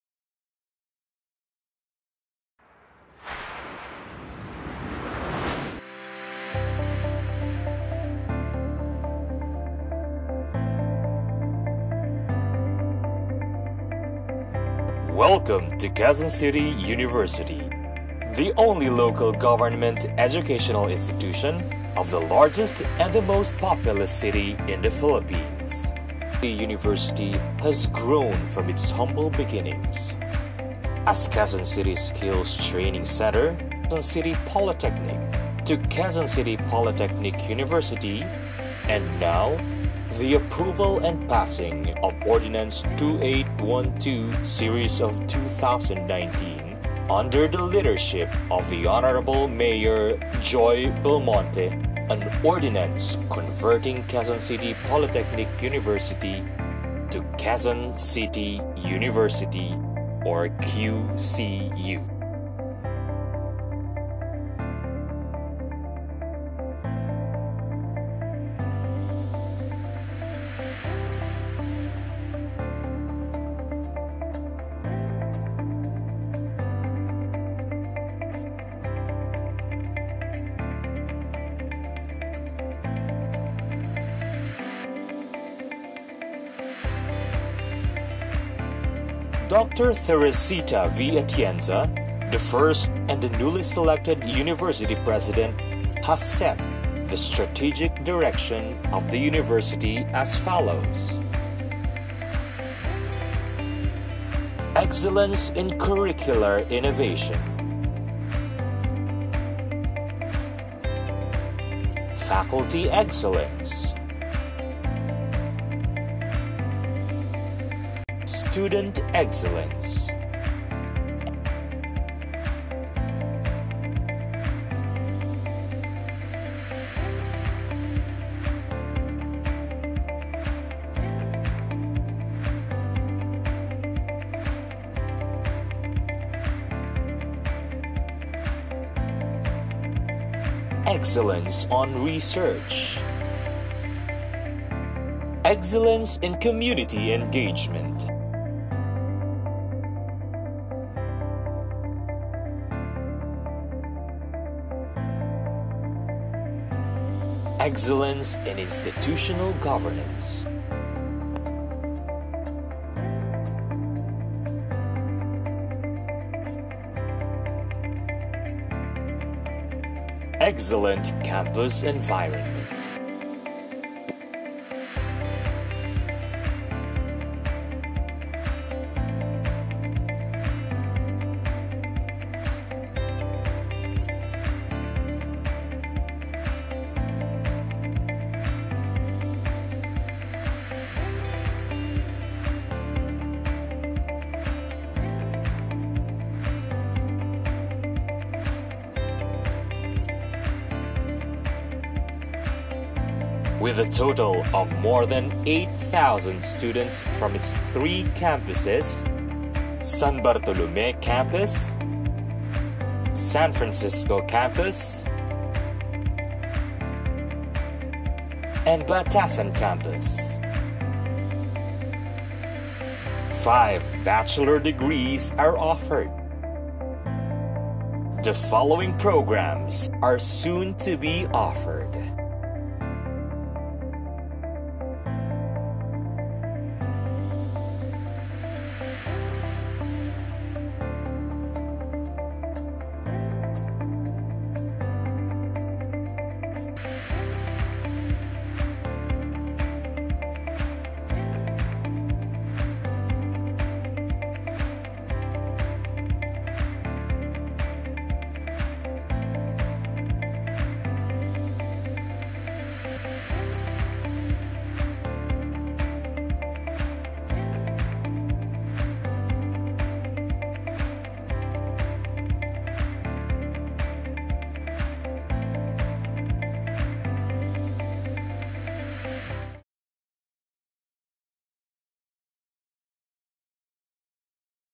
This is Quezon City University
Promotional Video of Quezon City University created April 30, 2021. In a presentation for the Virtual MoU Signing between Quezon City University and Miriam College. QCU President Dr. Theresita Atienza led today's virtual Memorandum of Understanding signing ceremony, alongside Miriam College's President, Ambassador Laura del Rosario. The QCU Research, Extension, Planning, and Linkages helped organize this agreement to further strengthen the instruction, research, and extension arms of the university and to complement our vision, mission, and goals.